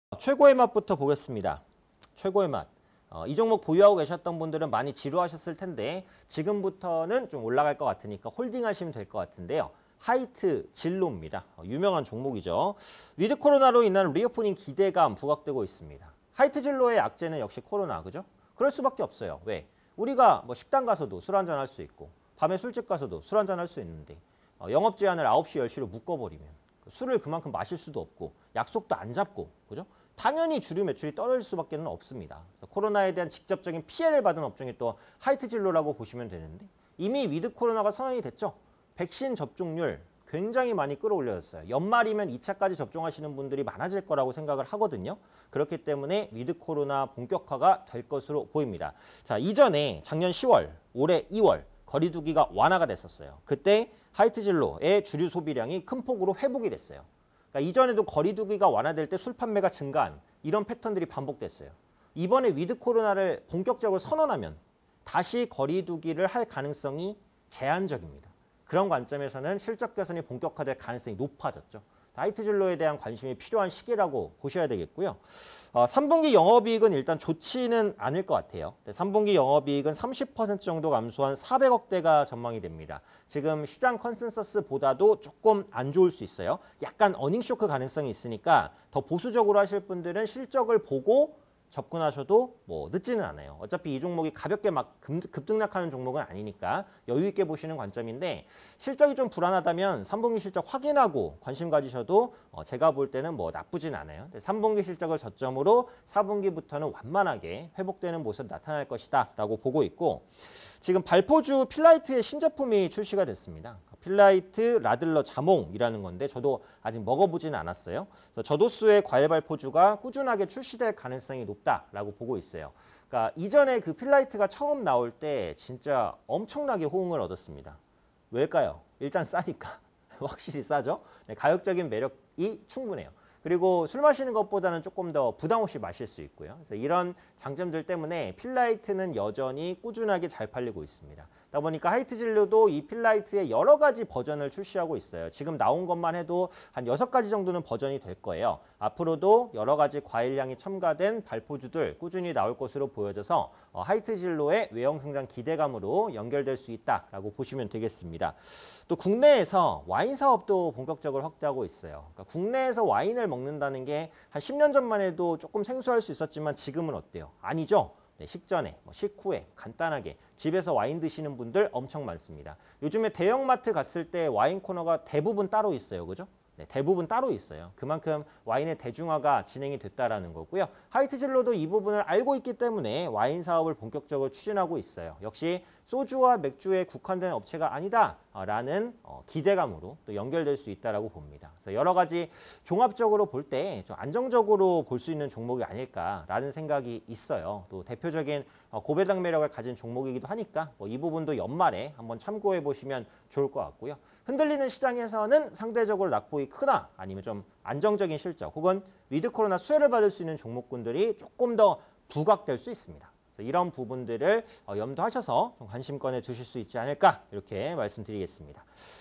하이트진로(000080)... ‘위드 코로나’로 인한 리오프닝 기대_최고의 맛_이데일리맵 오현진_런앤런 37회 (20211009)
하이트진로(000080)
- ‘위드 코로나’로 인한 리오프닝 기대
- 3Q 영업이익 시장 컨센서스 하회 전망
- 발포주 ‘필라이트’ 신제품 출시
- 국내 와인사업 확대 본격화